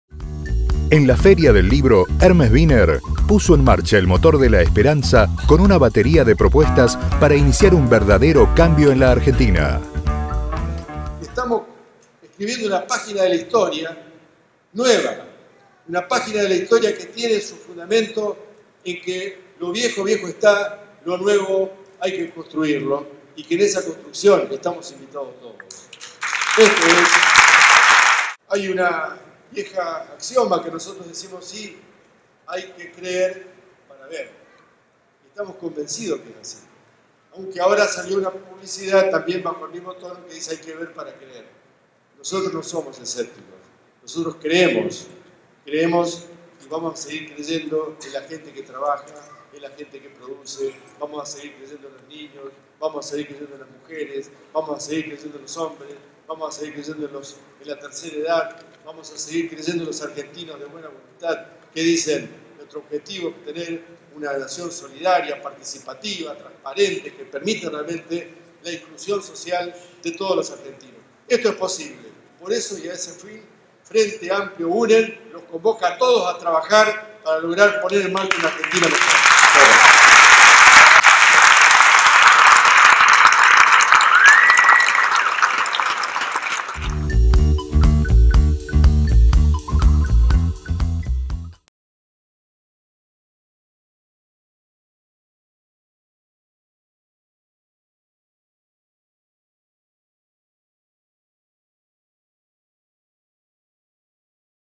BINNER PRESENTÓ SUS PROPUESTA PARA TRABAJAR POR LA ARGENTINA
En la Feria del Libro de Buenos Aires, Hermes Binner, presentó junto a Margarita Stolbizer y Humberto Tumini la publicación "Propuestas para trabajar por la Argentina que queremos", un compendio de iniciativas elaboradas a partir de recorridas por todas las economías y regiones del país. Este es un fragmento del discurso de Binner durante la presentación en la sala José Hernández.